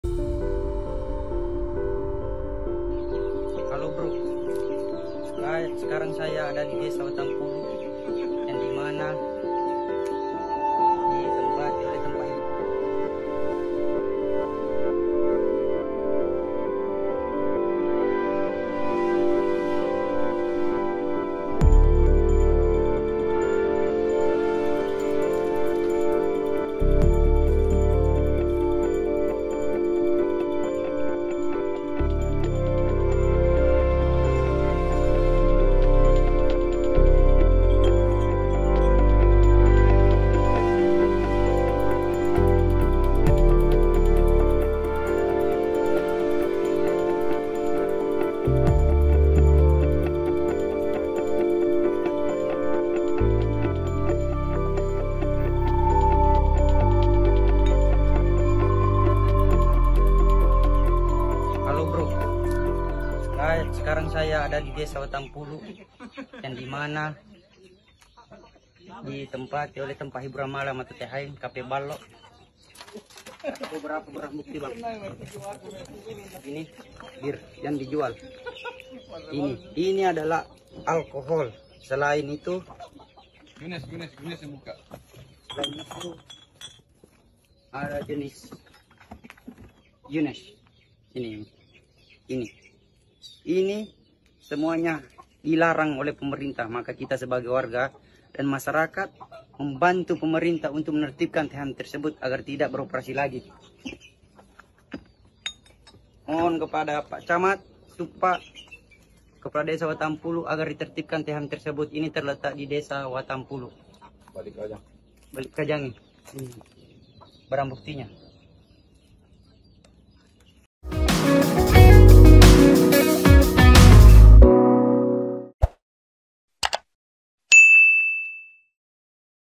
Kerap Terjadi Perkelahian, Warga Segel THM di Wattang Pulu Pinrang
Warga di Balikajangge, Desa Wattang Pulu, Kecamatan Suppa, Pinrang, mengambil tindakan dengan melakukan penyegelan Tempat Hiburan Malam (THM) yang terletak di sekitar pemukiman warga, Jumat (20/10).

Warga setempat telah lama merasa terganggu oleh kebisingan musik yang sangat keras dan seringnya terjadi perkelahian di sekitar lingkungan tersebut.

Tindakan penyegelan ini diambil sebagai respons terhadap keluhan tersebut.

Informasi yang diterima THM tersebut tidak memiliki surat izin resmi untuk beroperasi.

Sebuah video juga menampilkan seorang remaja yang memperlihatkan kondisi THM setelah proses penyegelan dilakukan.
Video terkini dan terbaru..